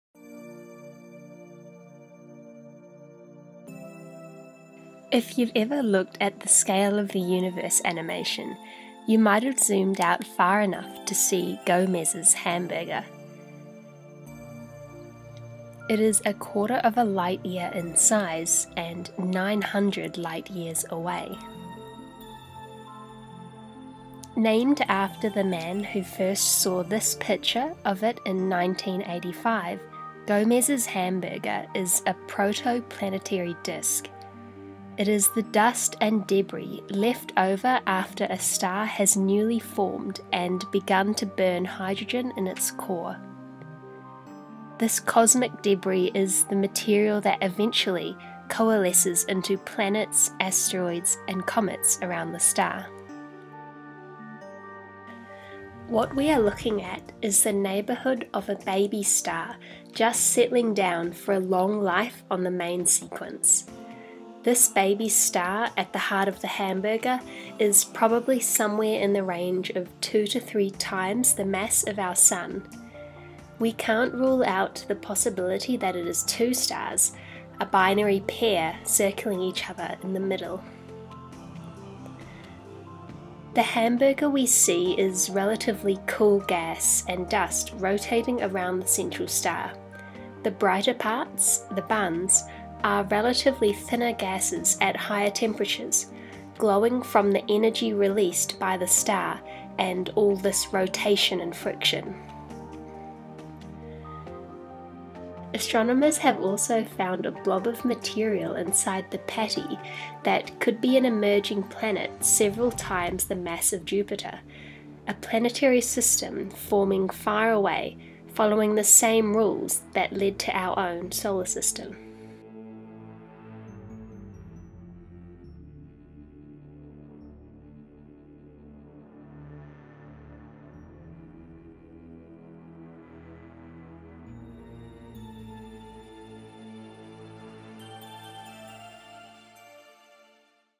Gomez's Hamburger
If you’ve ever looked at The Scale of the Universe Animation you might have zoomed out far enough to see Gomez’s Hamburger. It is a quarter of a light year in size and 900 light years away.

Named after the man who first saw this picture of it in 1985, Gomez's Hamburger is a protoplanetary disk. That is dust and debris left over after a star has newly formed and begun to burn hydrogen in its core. This cosmic debris is the material that eventually coalesces into planets, asteroids and comets around the star.

Credit: Arturo Gomez (CTIO/ NOAO), Hubble Heritage Team, NASA